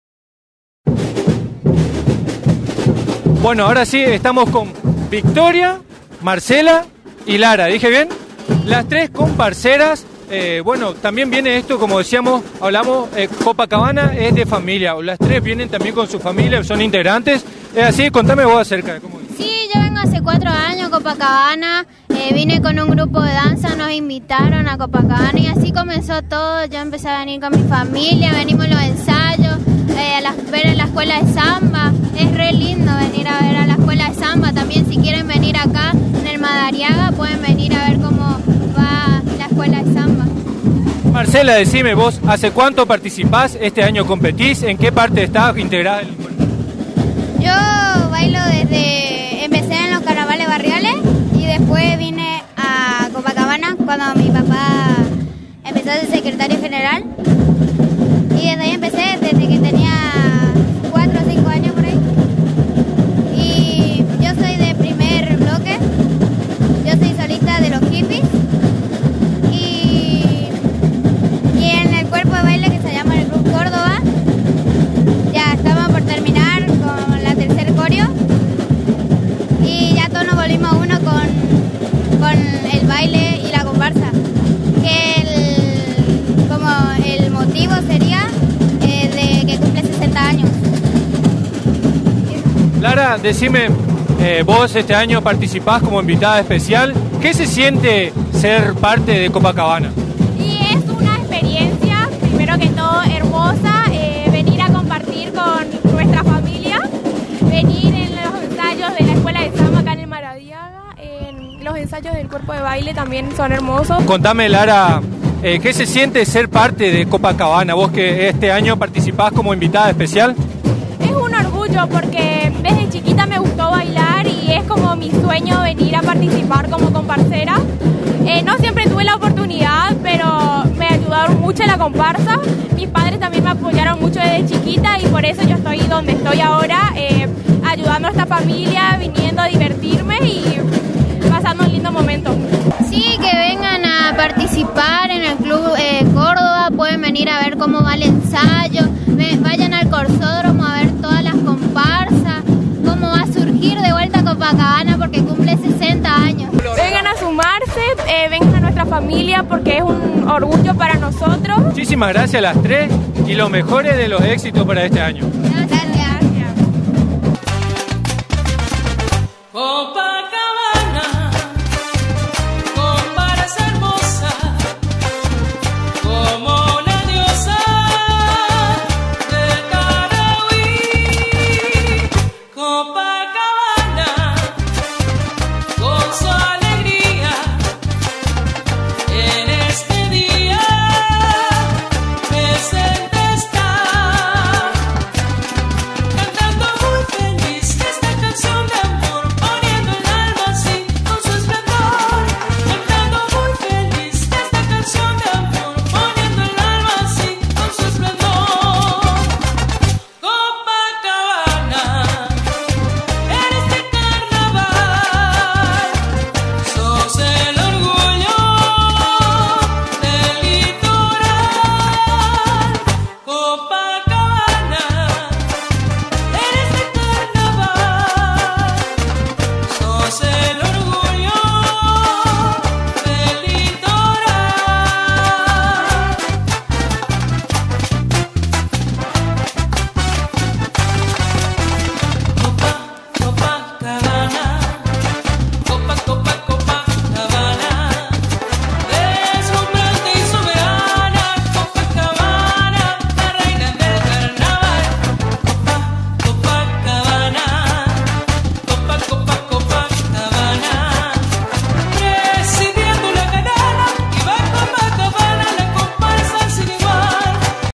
COPACABANA 2020 COMPARSERAS
ENTREVISTAMOS A LARA RIVERO INVITADA ESPECIAL, MARCELA CORREA Y VICTORIA EN EL ENSAYO PREPARANDOSE PARA LOS 60 AÑOS DE COPACABANA.